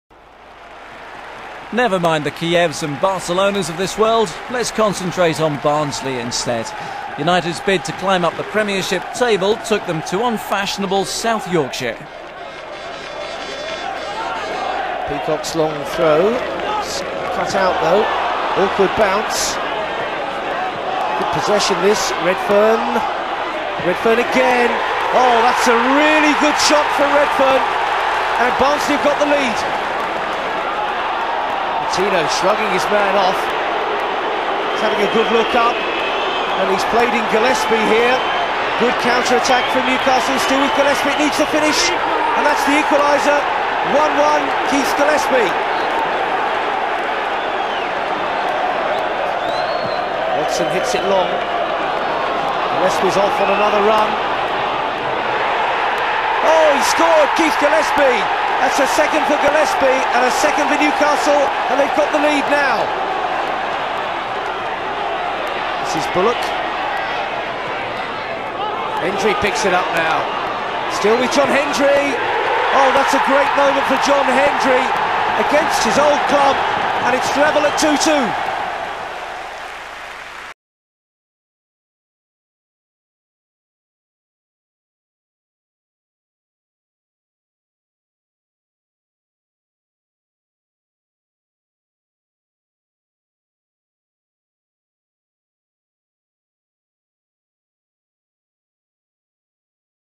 Barnsley v Newcastle United 1997-98
Highlights of Barnsley v Newcastle from the English Premier League during the 1997/98 season.